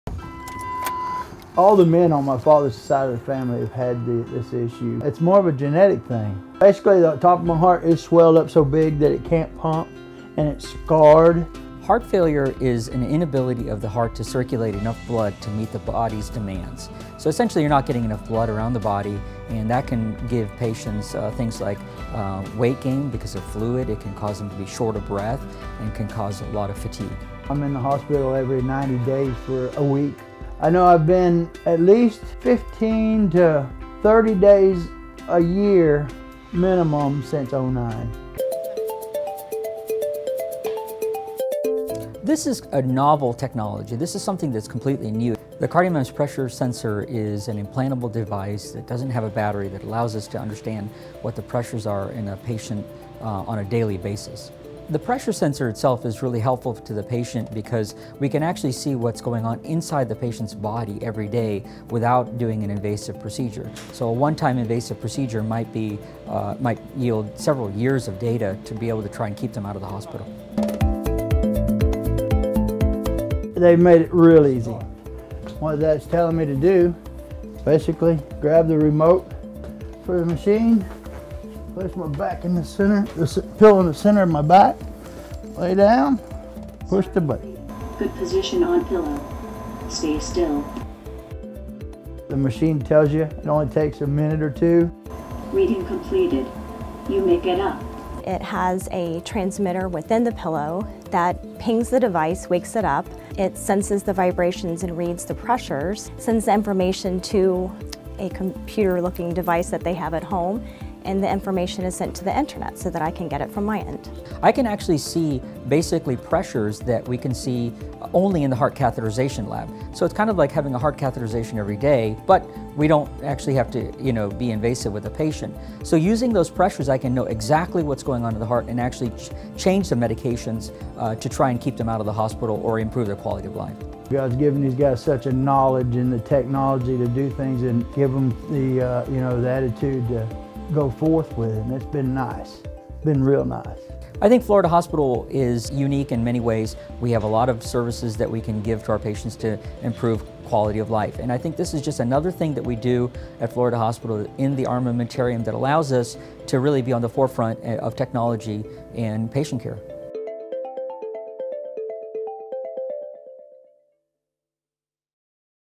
Florida Hospital Patient is First in Central Florida to Receive New Heart Failure Monitoring Device
John Kinser is the first patient in Central Florida and among the first in the state to receive the CardioMEMS HF System, a wireless sensor that allows physicians to monitor their patient’s heart remotely.  CardioMEMS is the first FDA-approved heart failure monitoring device that has been proven to significantly reduce hospital admissions when used by physicians to manage heart failure.